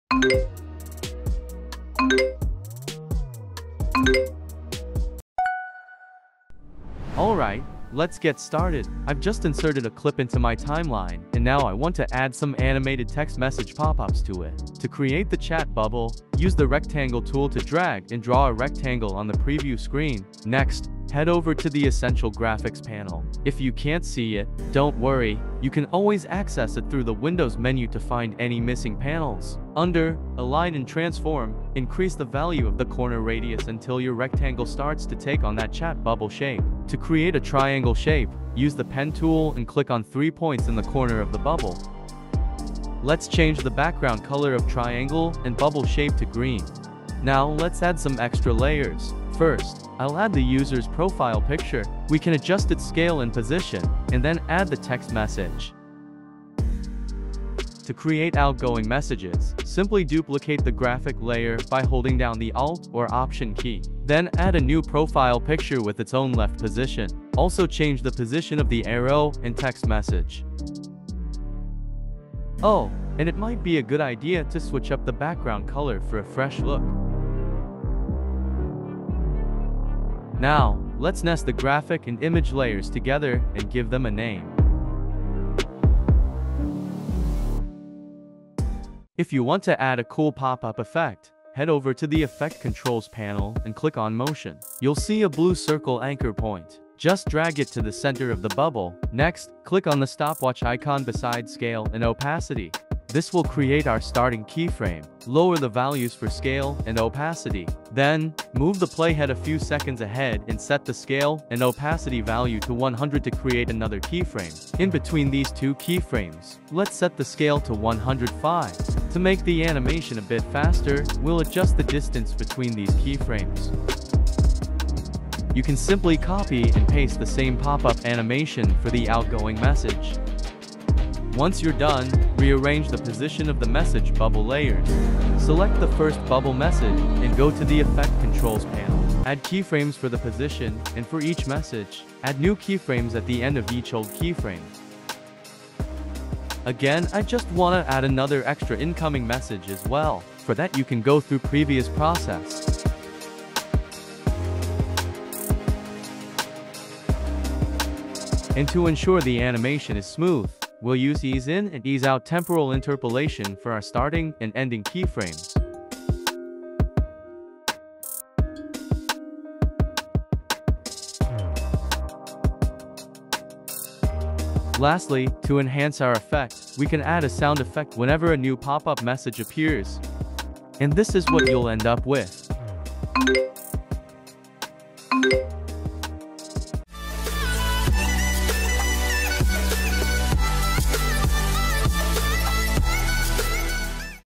Create TEXT MESSAGE Popup with SOUND Effect in Premiere Pro
In this video, I'll show you how to create a realistic text message popup with a sound effect in Adobe Premiere Pro. This effect is perfect for adding a touch of realism to your videos, whether you're creating a vlog, a tutorial, or a short film.

Timestamps:
00:00  Intro
00:07  Add Text Message
01:29  Add Popup Effect
02:11  Add Slide Up Effect
02:54  Add Sound Effect
03:00  Final result

Resource:
🎬Video by cottonbro studio: